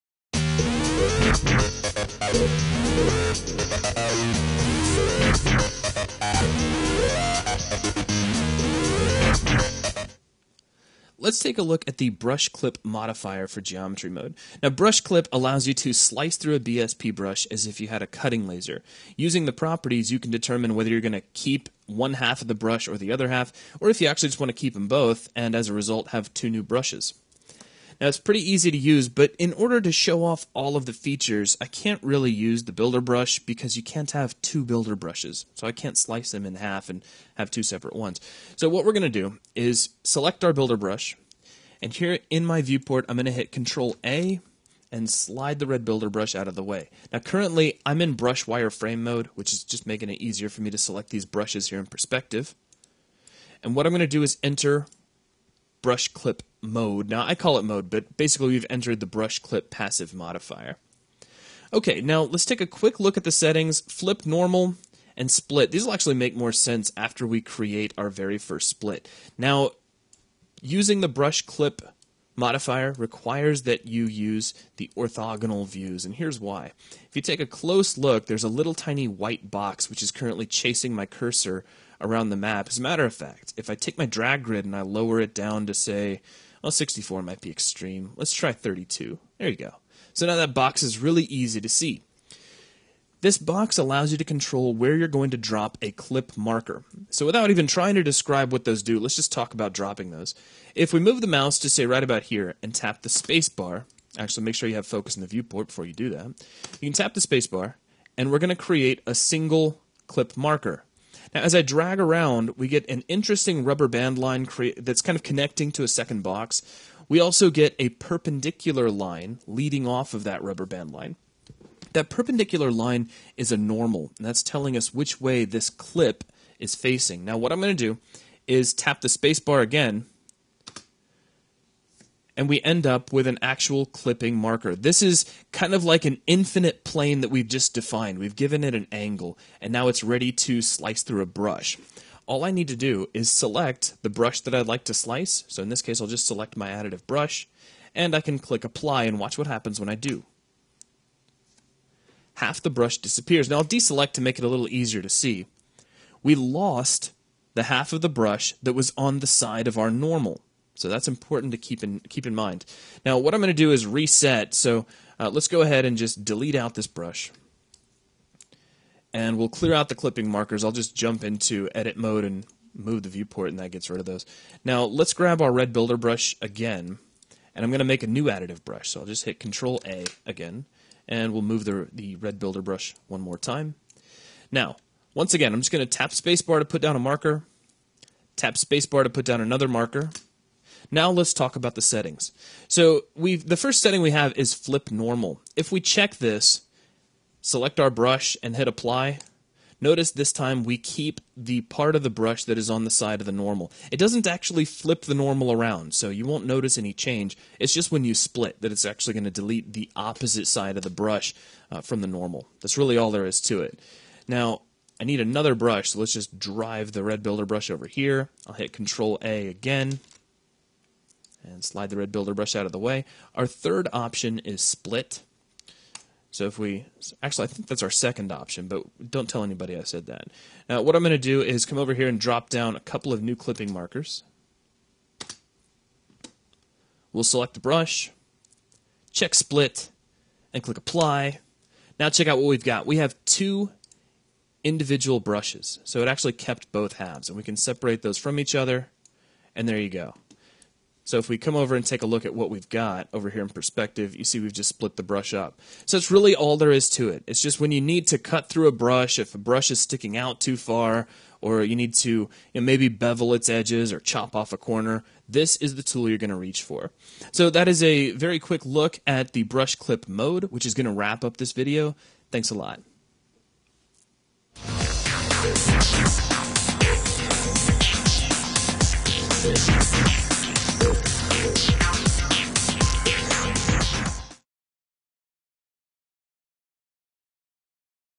UDK Tutorial Part 69: Geometry Mode- Brush Clip
Brush Clipping allows for brushes to be chopped up, as if with a cutting laser. This video shows how to clip your brushes